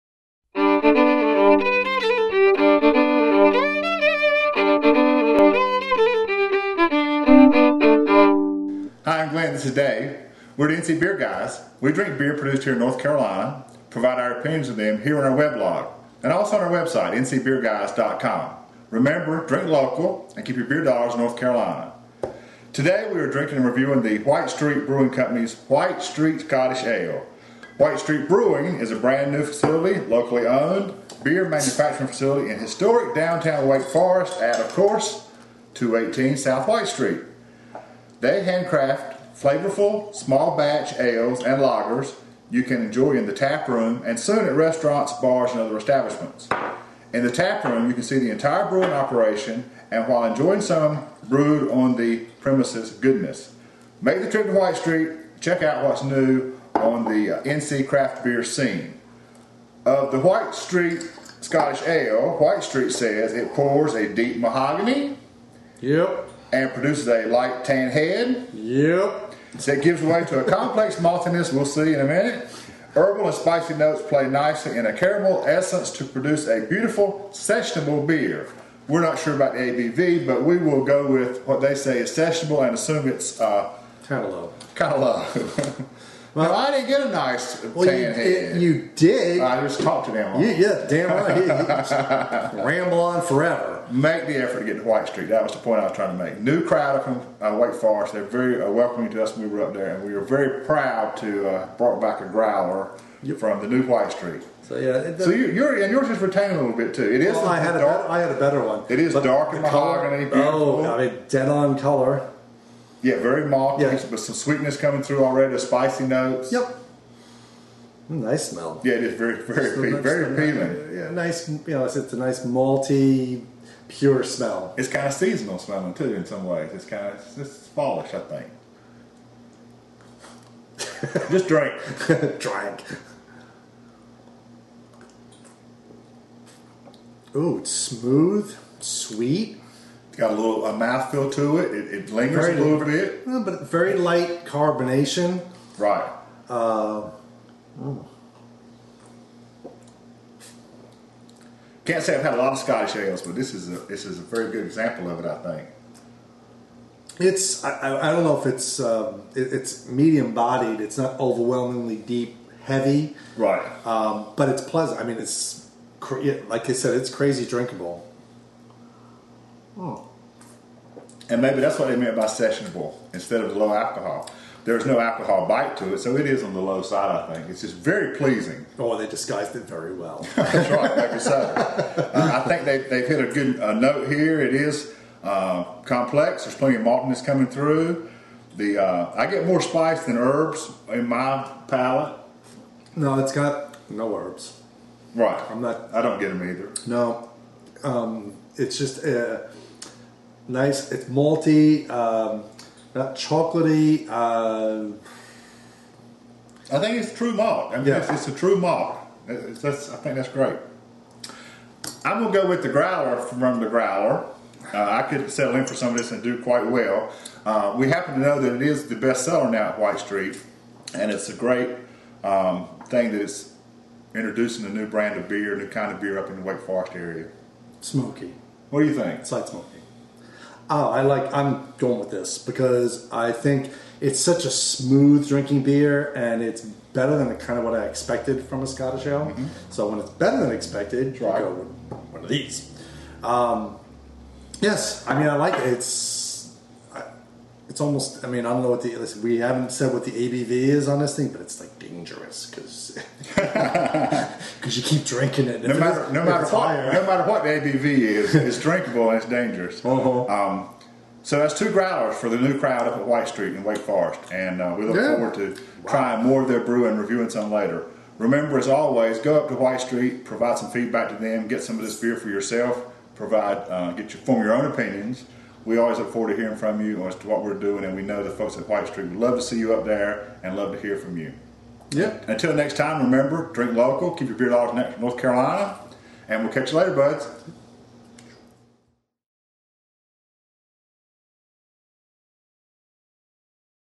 Episode #59 - White Street Brewing Co. - Scottish Ale
Having finally made it up to the new White Street Brewing Co. located in historic downtown Wake Forest, the NCBeerGuys came home with a growler of White Street Scottish Ale for today's review! White Street says it pours "a deep mahogany ... produces a tight tan head that gives way to a complex maltiness. Herbal and spicy notes play nicely in a caramel essence to produce a beautifully sessionable beer." There's no better way to enjoy it than straight from the factory. Let's see how nice it plays with the NCBeerGuys!